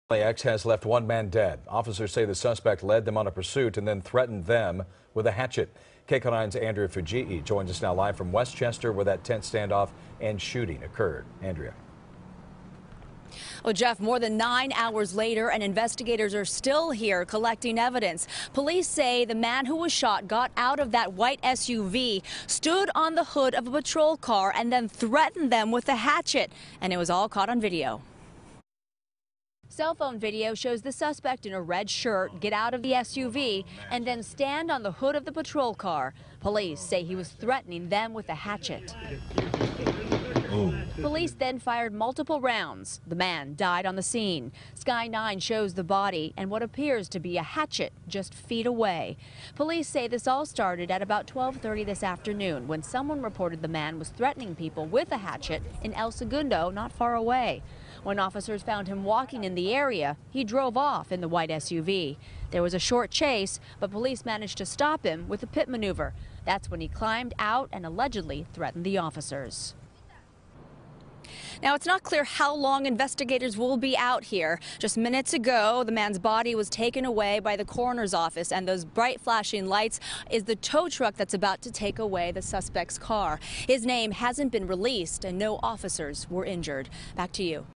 Man Wielding Hatchet Shot, Killed After Chase, Authorities Say
According to the Los Angeles County Sheriff’s Department, the suspect jumped onto the hood of a police cruiser while holding the hatchet just prior to being shot. Andrea Fujii reports.